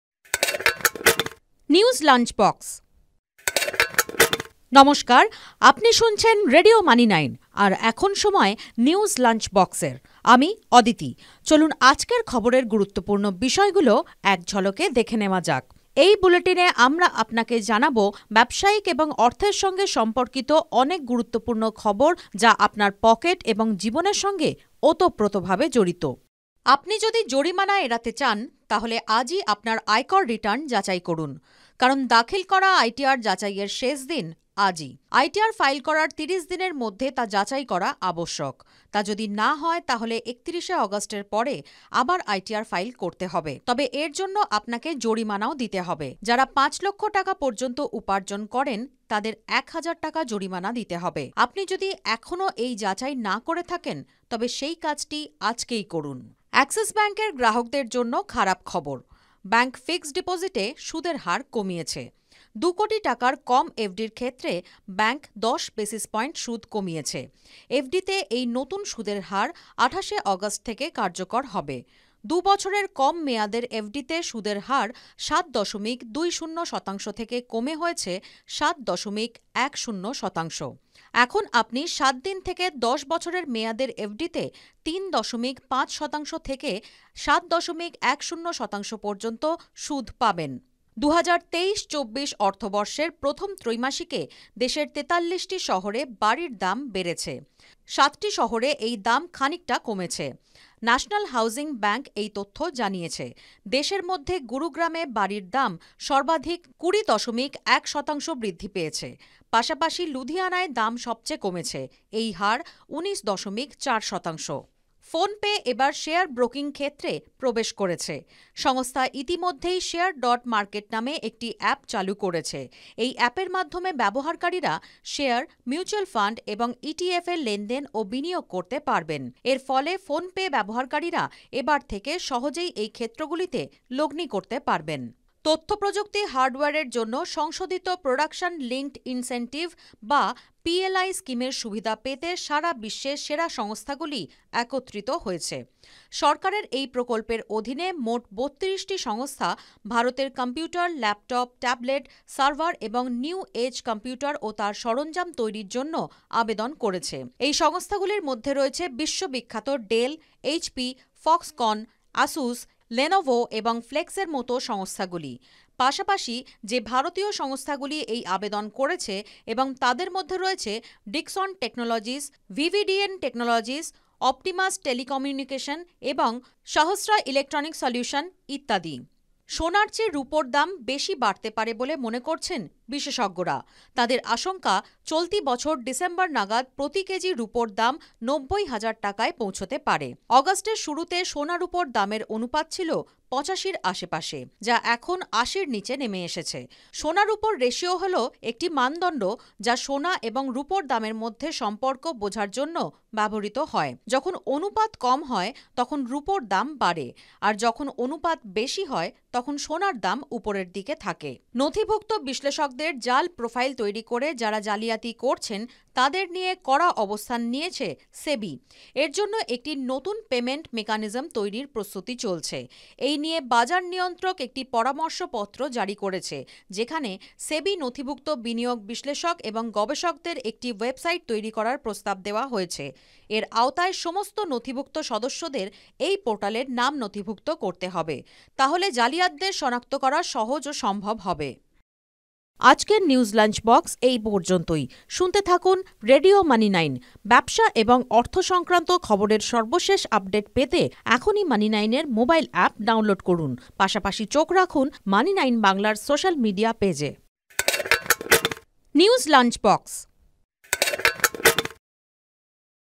podcast আজই ITR যাচাইয়ের শেষ দিন
2023-24 অর্থবর্ষের প্রথম ত্রৈমাসিকে দেশের 43টি শহরে বাড়ির দাম বেড়েছে। সাতটি শহরে এই দাম খানিকটা কমেছে। ন্যাশনাল হাউজিং ব্যাঙ্ক এই তথ্য জানিয়েছে। দেশের মধ্যে গুরুগ্রামে বাড়ির দাম সর্বাধিক 20.1% বৃদ্ধি পেয়েছে। পাশাপাশি, লুধিয়ানায় দাম সবচেয়ে কমেছে, এই হার 19.4%।

PhonePe এবার শেয়ার ব্রোকিং ক্ষেত্রে প্রবেশ করেছে। সংস্থা ইতিমধ্যেই 'Share.Market' নামে একটি অ্যাপ চালু করেছে। এই অ্যাপের মাধ্যমে ব্যবহারকারীরা শেয়ার, মিউচুয়াল ফান্ড এবং ইটিএফ-এ লেনদেন ও বিনিয়োগ করতে পারবেন। এর ফলে PhonePe ব্যবহারকারীরা এবার থেকে সহজেই এই ক্ষেত্রগুলিতে লগ্নি করতে পারবেন।